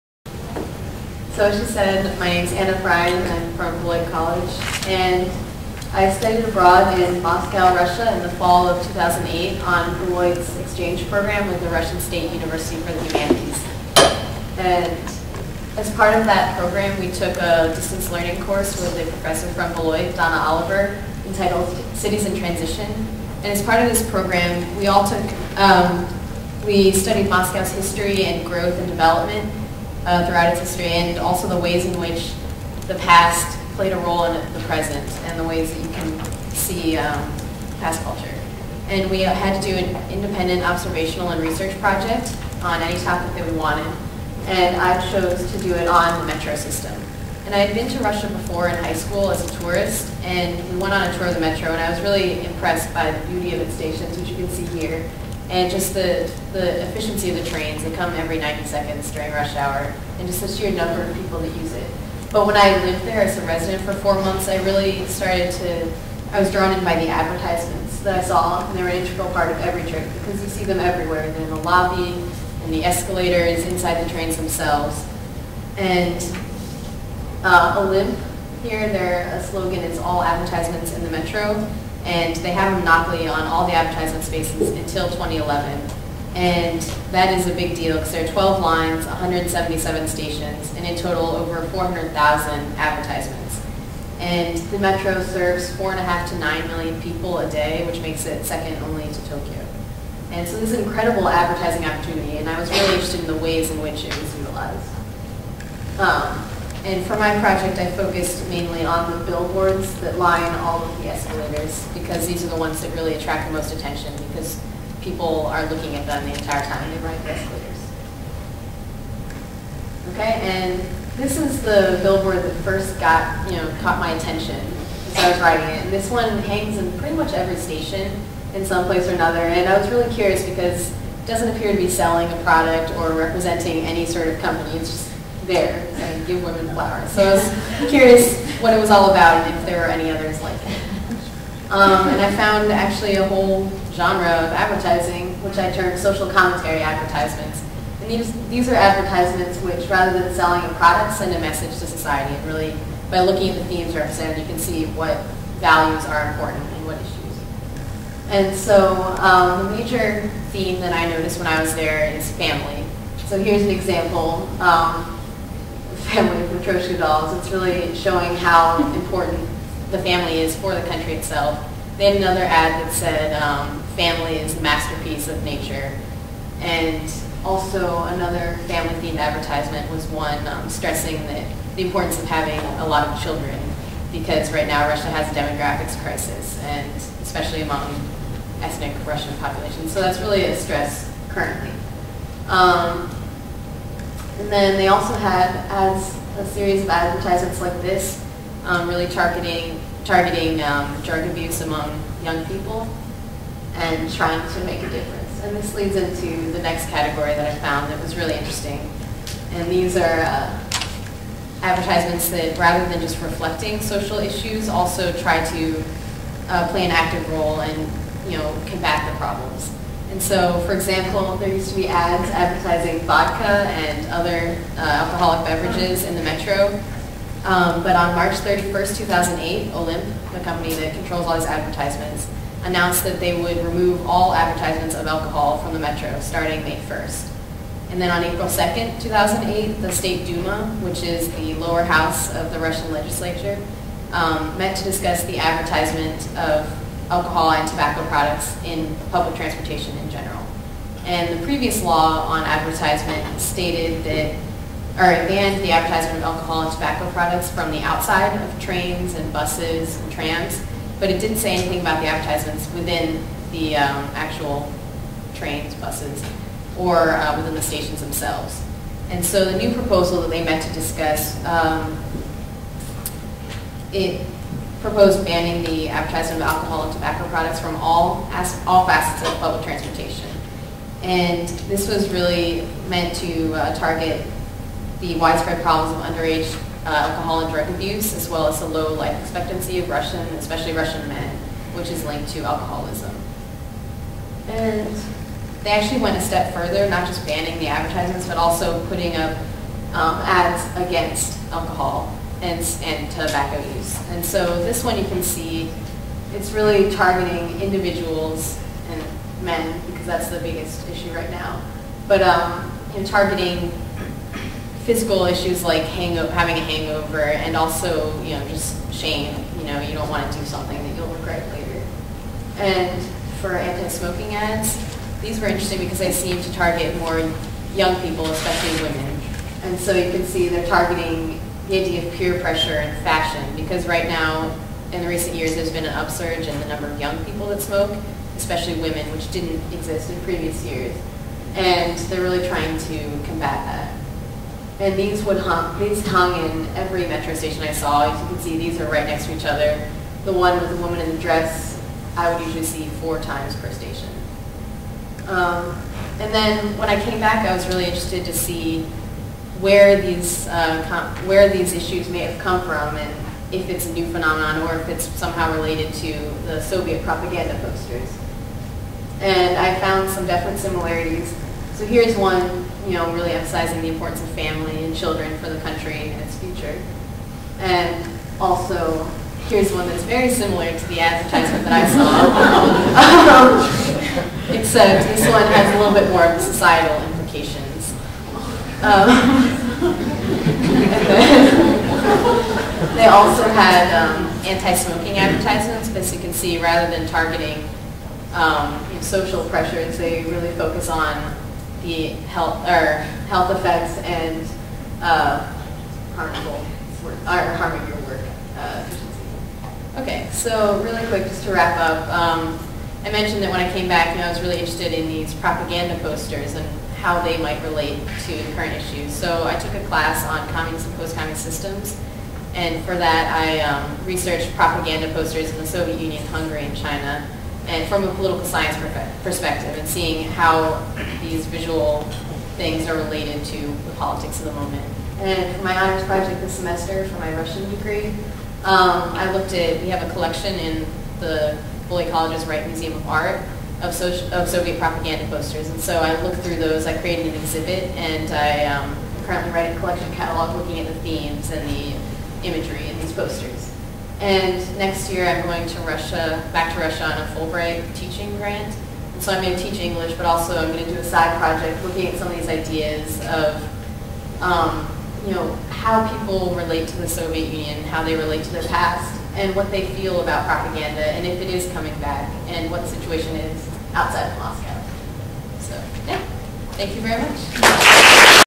2010 ACM Student Symposium - Anna Bryan, Beloit College
Recorded April 16, 2010 at the ACM Student Symposium on Off-Campus Study in Chicago, IL.  Anna Bryan's presentation is titled, "Engaging the Social Commentary Advertisements in the Moscow Metro: Modern Day Propaganda?"